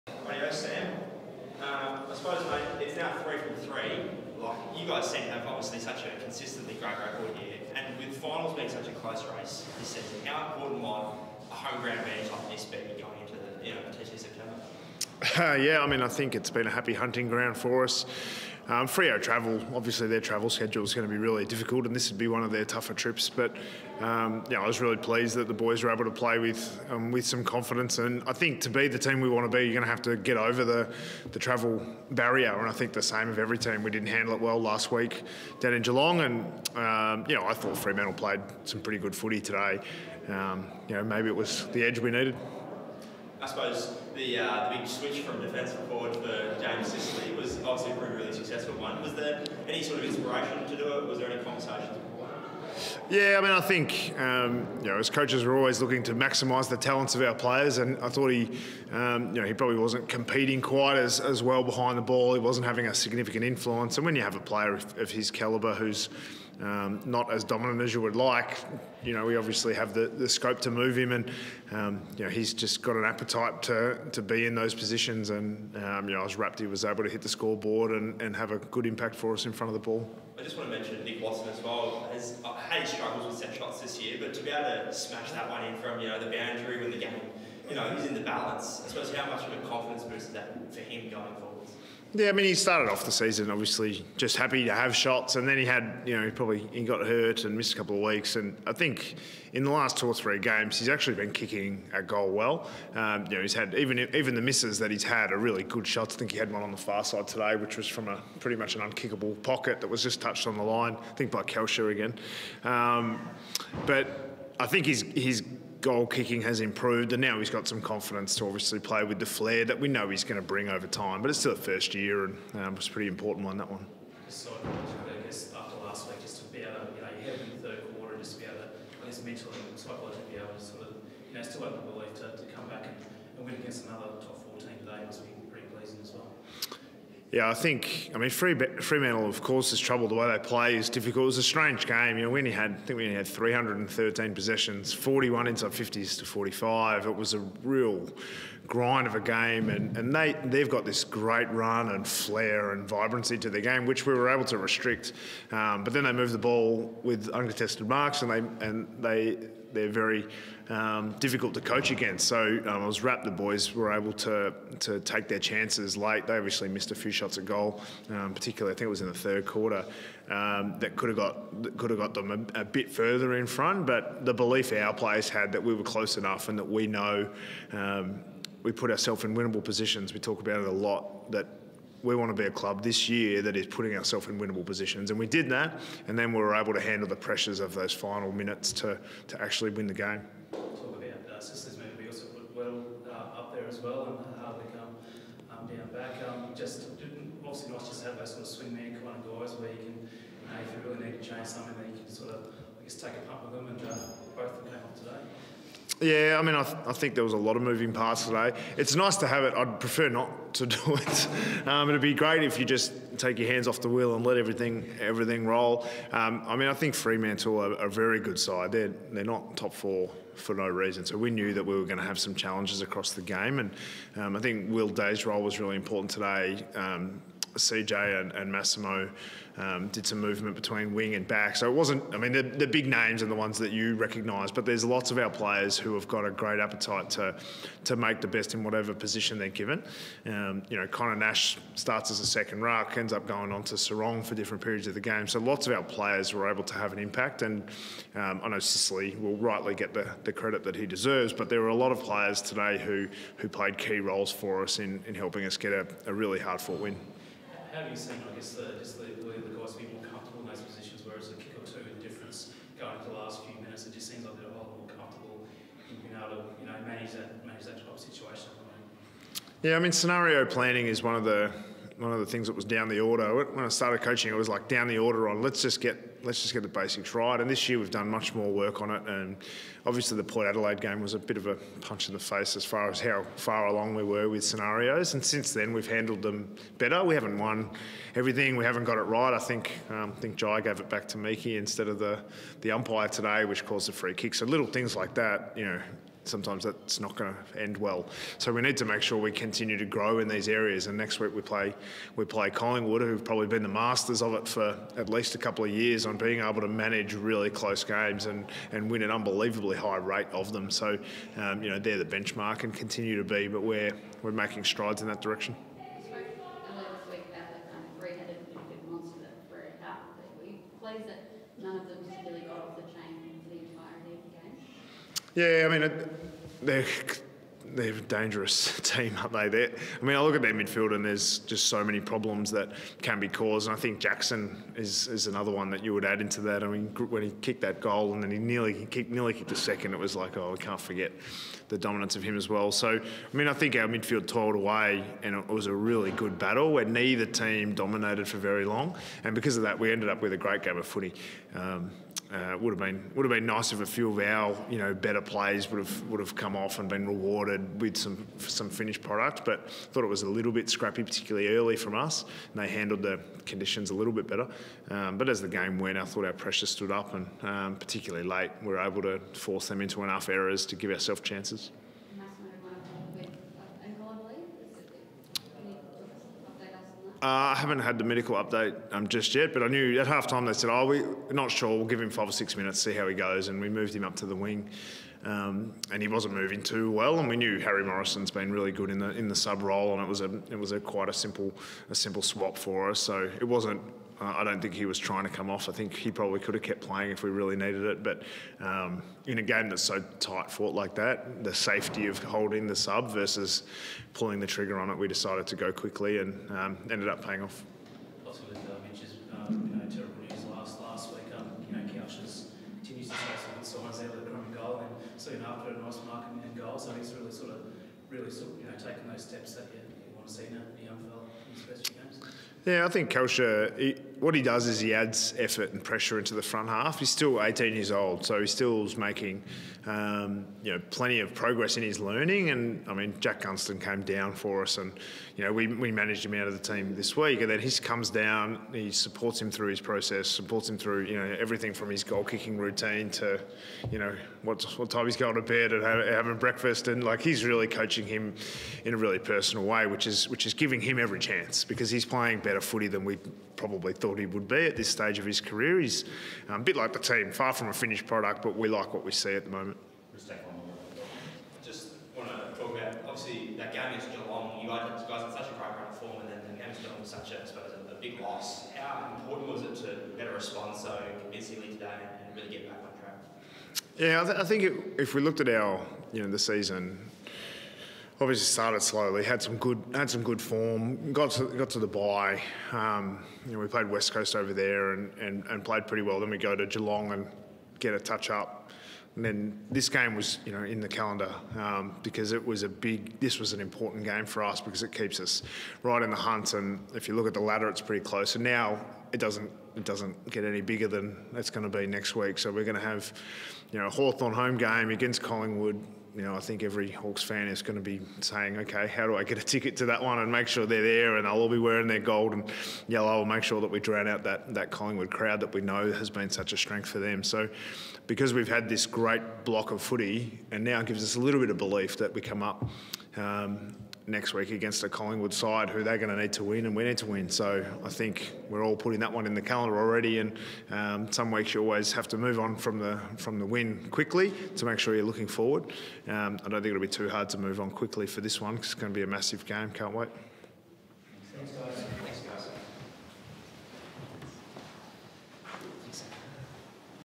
Mitchell on the HEART POUNDING win against the Dockers | Press Conference
Go Hawks!